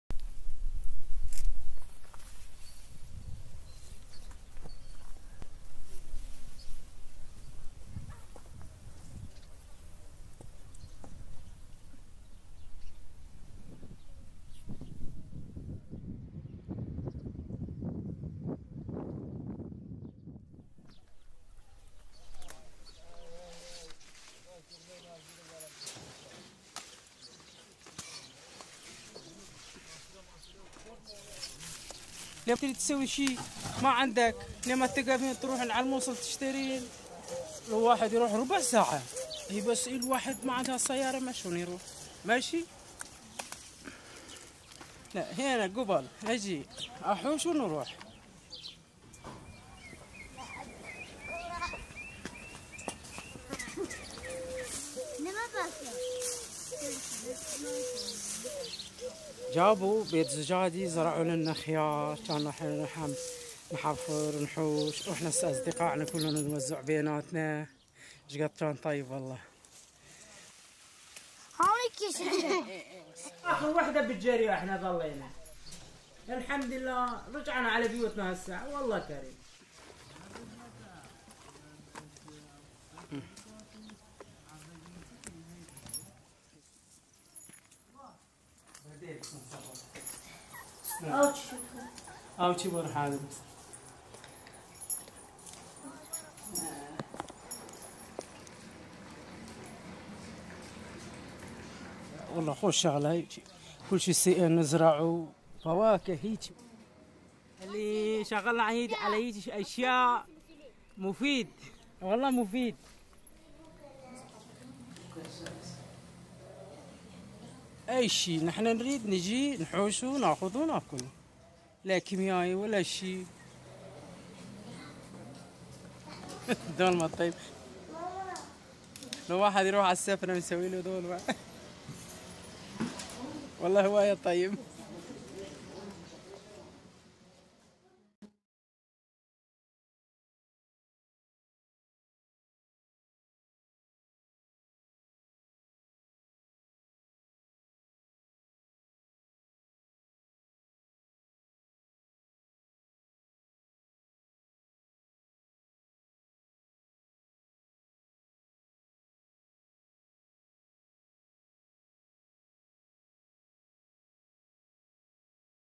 How kitchen gardening can make a difference in Northern Iraq
Mahdia laughs in her garden in a village in Northern-Iraq. She has seen it grow from scratch since she started participating in 2017 in a kitchen garden activity - part of a broader project by People in Need, Yazda Organization and the World Food Programme (WFP); funded by the Governments of Australia, Canada, Germany, Japan, Switzerland.